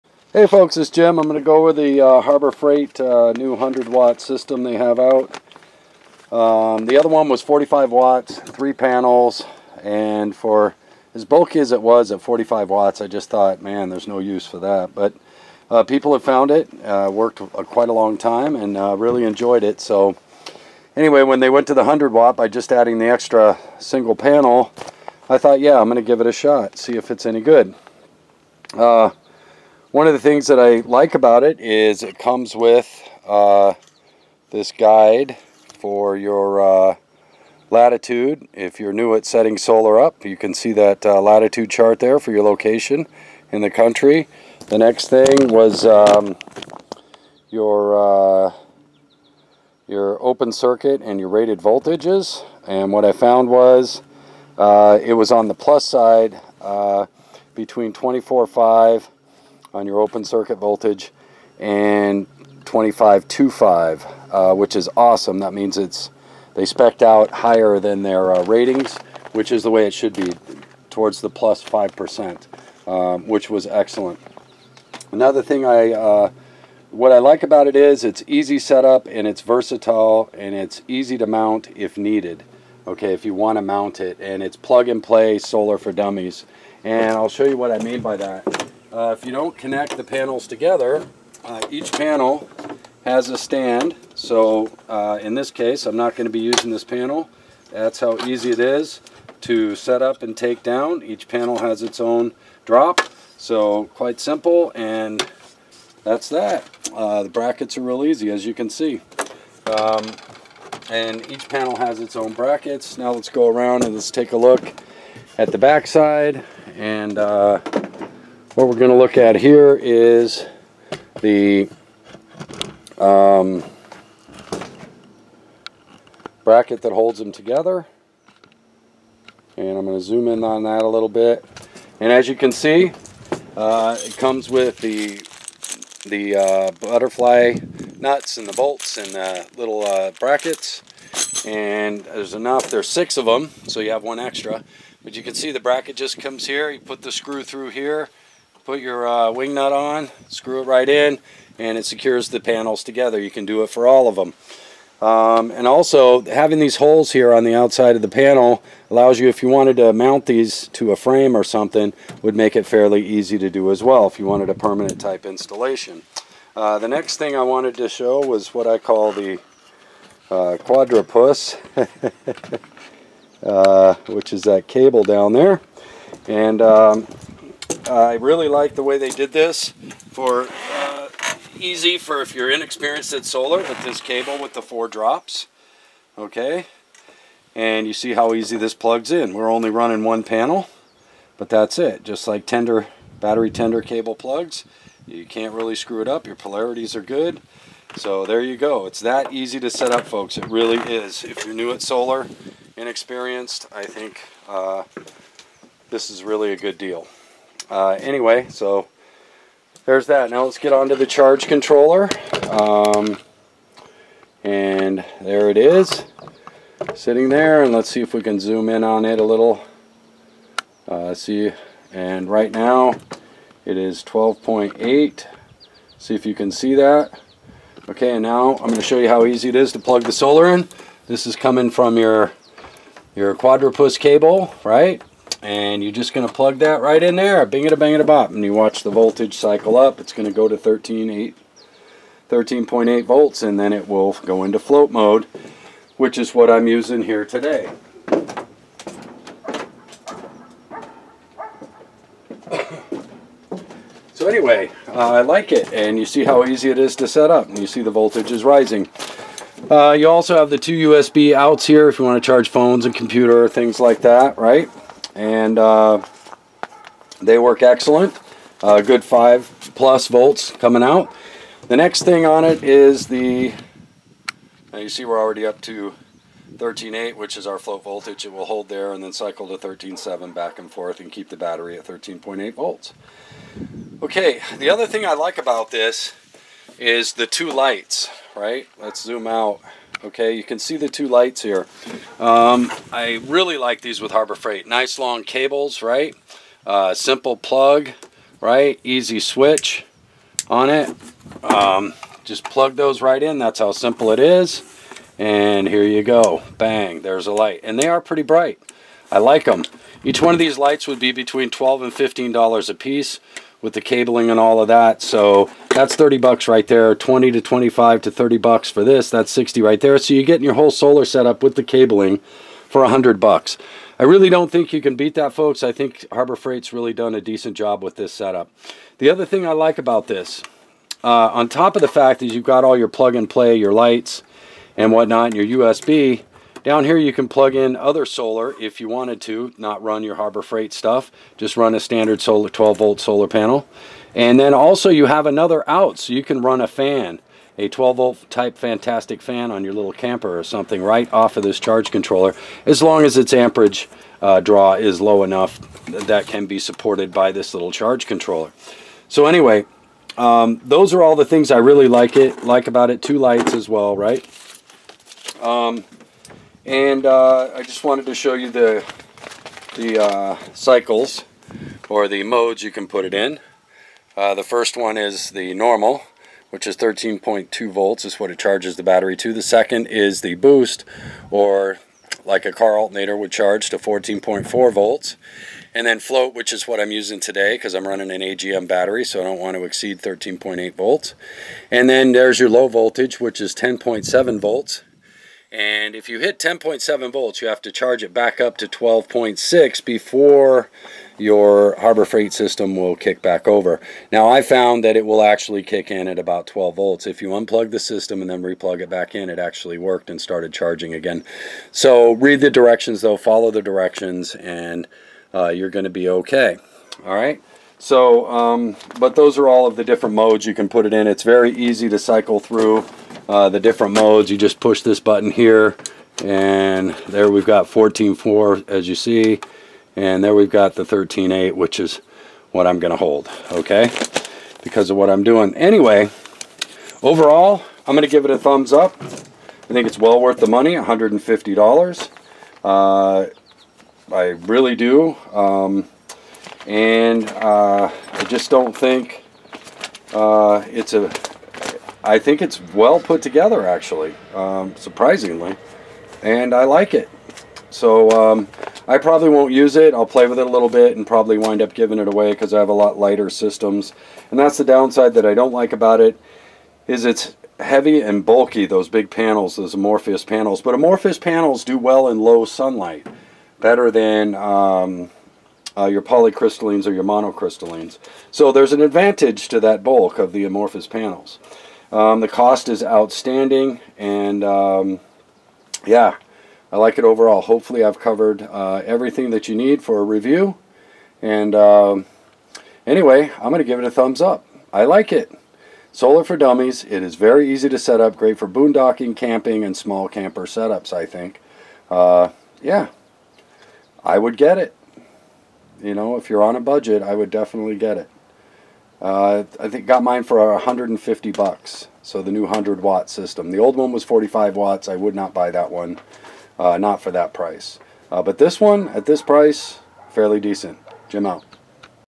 Harbor Freight 100W solar system review
This is a decent little system for $149.99 with the super coupon.  It comes with 2 lights and is easy to set up.  Solar for dummies if you will.  Easy connections that help to prevent reverse polarity.